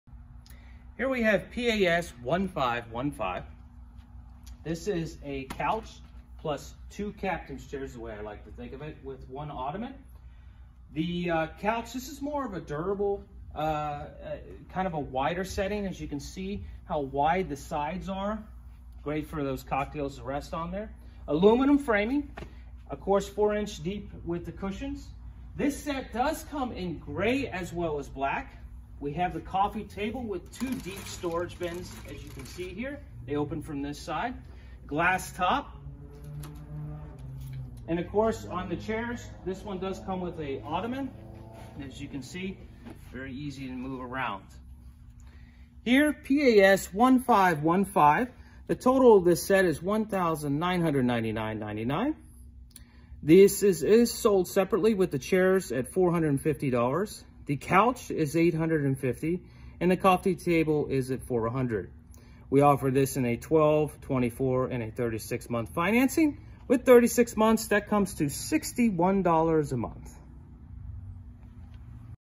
Wicker Pocahontas 5pc Patio Garden Furniture Sofa Set Sectional
This deluxe dining set with 4 curved chairs offers supreme comfort, grandeur and sophistication for your garden. Made with the highest quality hand-woven rattan, the MacArthur dining set comes complete with tempered glass and seat cushions.